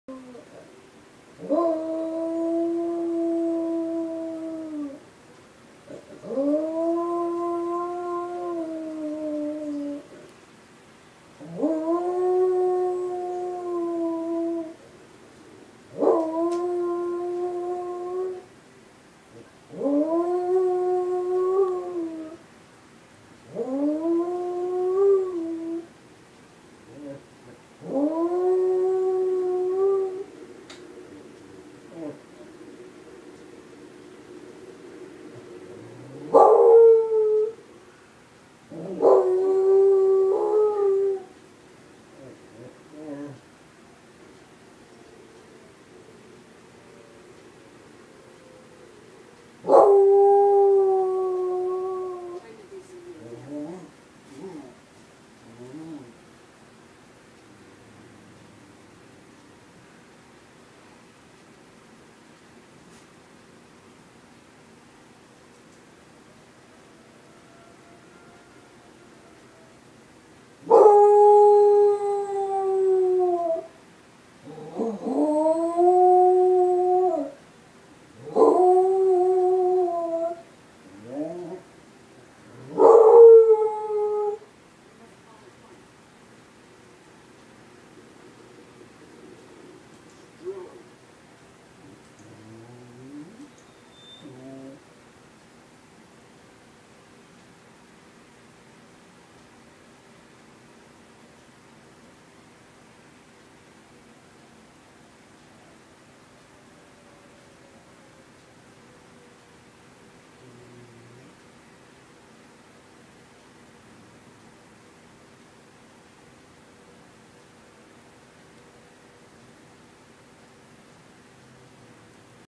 Beagle Emergency Siren
Every time my dog hears the Emergency Siren, he has to get in on the Act.